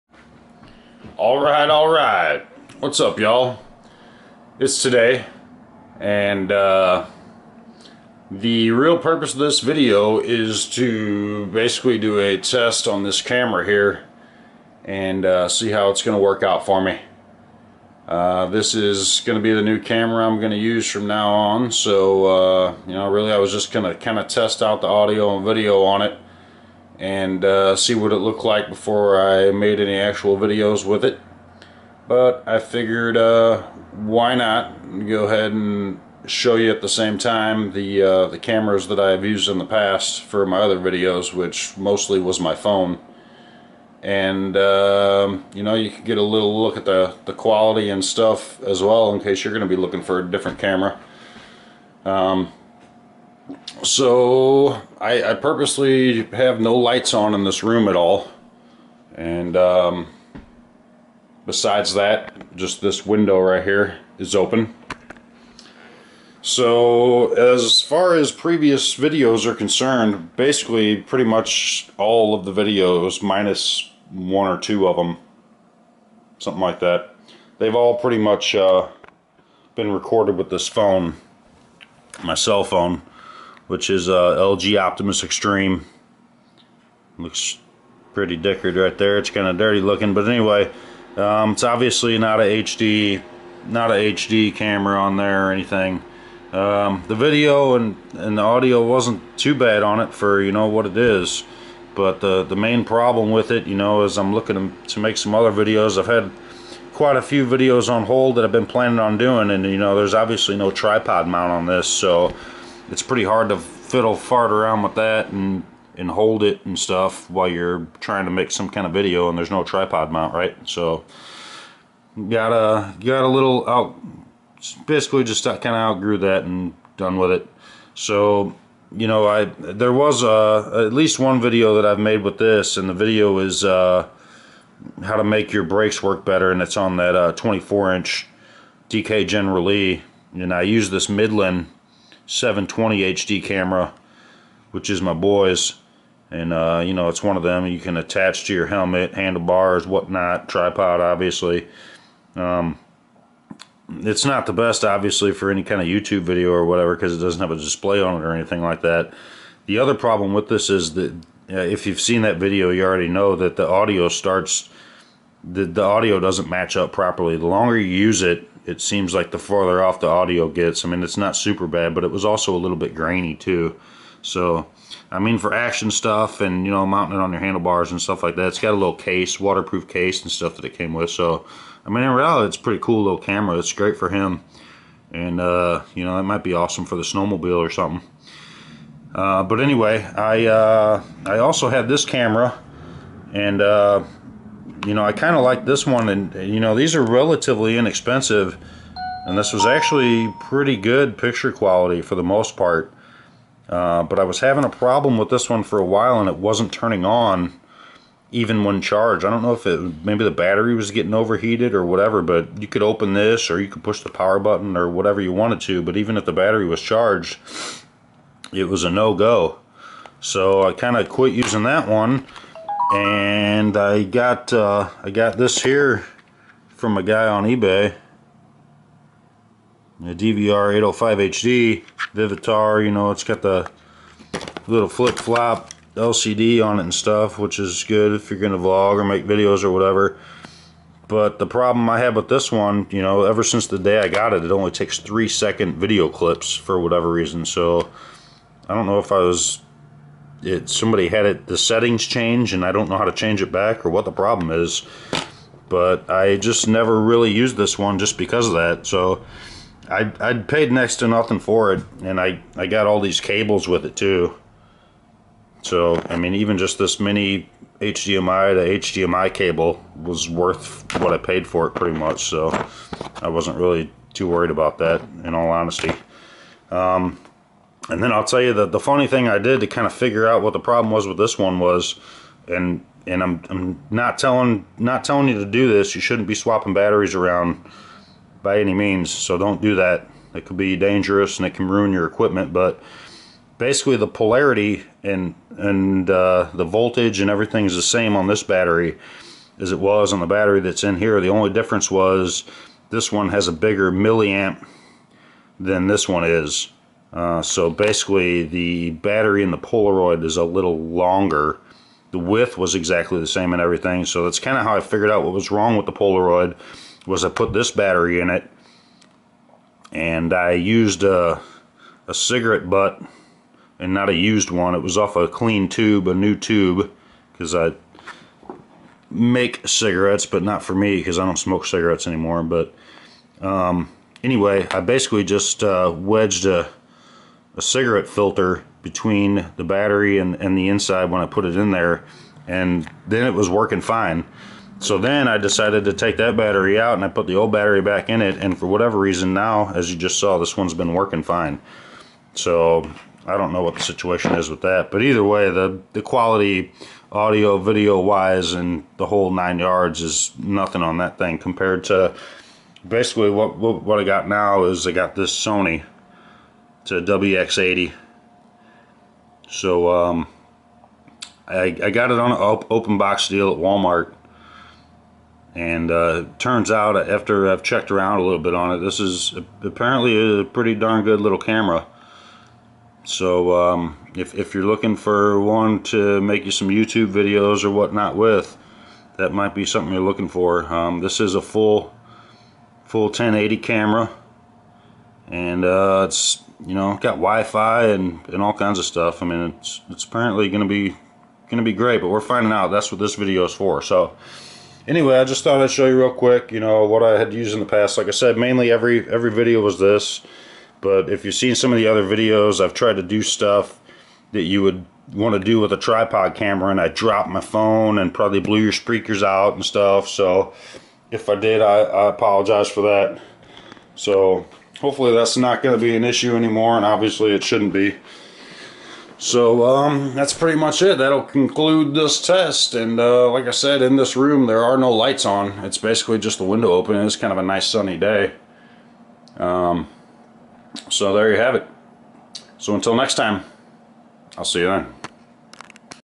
Test run of the Sony DSC-WX80 Cyber-shot
Basically a audio and video test for the Sony WX80 Cyber-shot point and shoot.. and some previous cams.